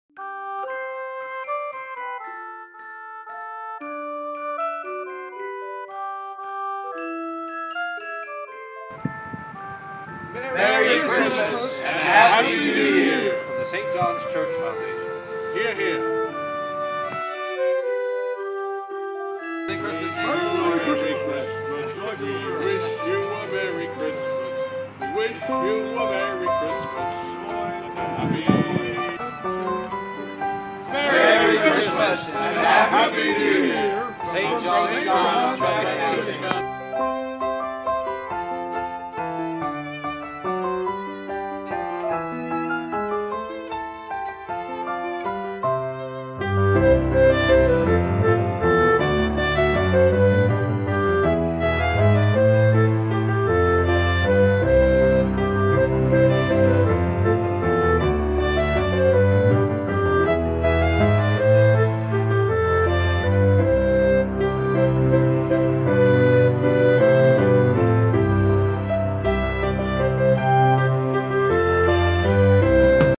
Merry Christmas from George Washington, Patrick Henry, Thomas Jefferson and more!
We wish you a Merry Christmas! George Washington, Thomas Jefferson, Patrick Henry and the rest of the delegates from the Second Virginia Convention of 1775 wish you the best! A short video by the St. John's Church Foundation, a non-profit organiation, dedicated to preserving historic St. John's Church. St. John's Church was built c. 1741 and is a National Historic Landmark. It was the site of Patrick Henry's "Liberty or Death" speech at the Second Virginia Convention on March 23, 1775. The foundation works to preserve the church and provide educational programming about the legacy of liberty.